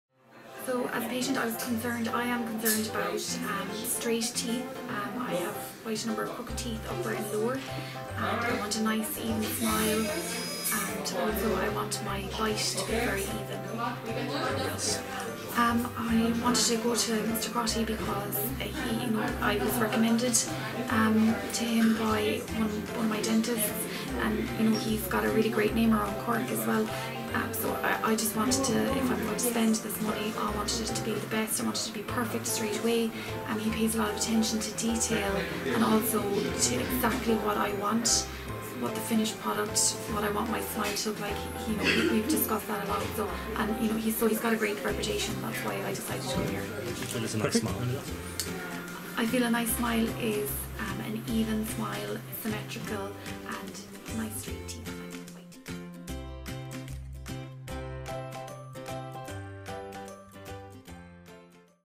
Trust is a big part of a new smile
Joanne is starting her new smile journey. Trust has played a big part in her selecting Dr Owen Crotty in Cork to be her Smile Specialist.